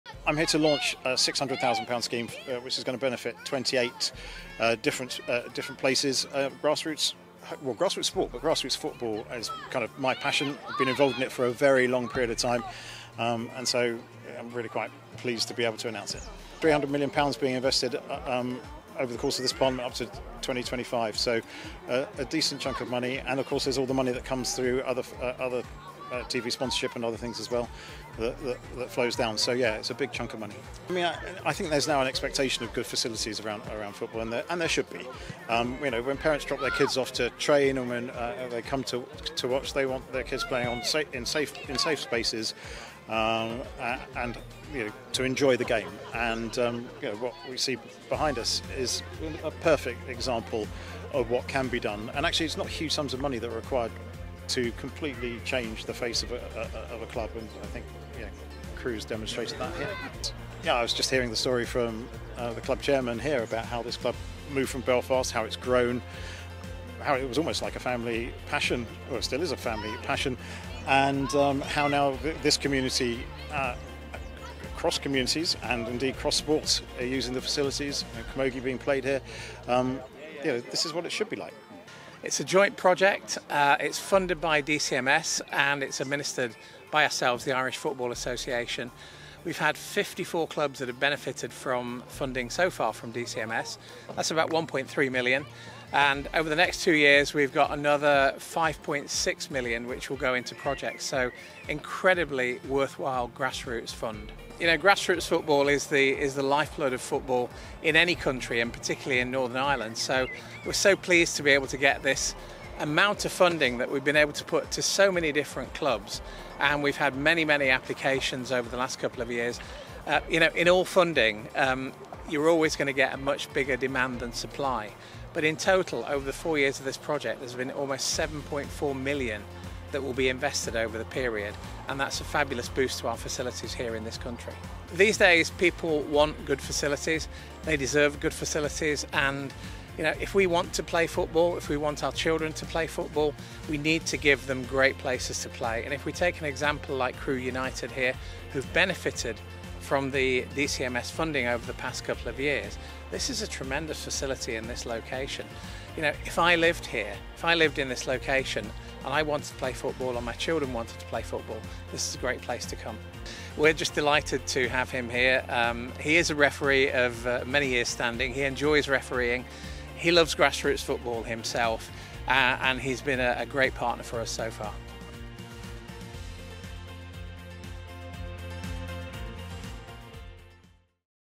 Irish FA DCMS fund | Secretary of State for Northern Ireland visits Crewe United
Secretary of State for Northern Ireland Chris Heaton-Harris visits Crewe United as part of Irish FA DCMS fund rollout